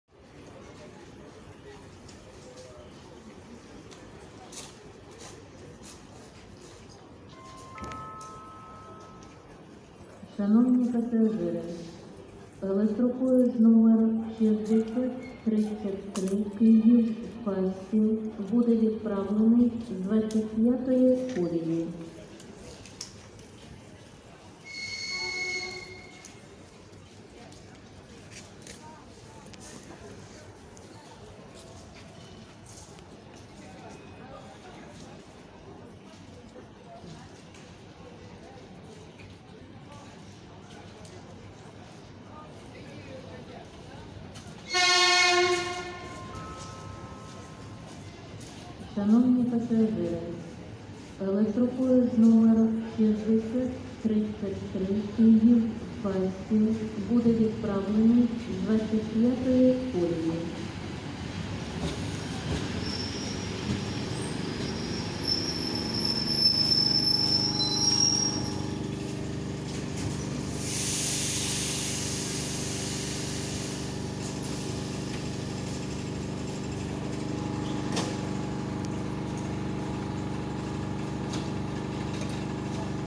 "+20 минут опоздания" эпл9т-006 6032 Житомир-киев+толпа на 6033 Киев-Фастов +26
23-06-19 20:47

"+20 минут опоздания" Шестивагонная эпл9т-006 №6032 Житомир-Киев и толпа на 6033 Киев-Фастов +26 минут опоздание. Вдали поломанная эпл9т-007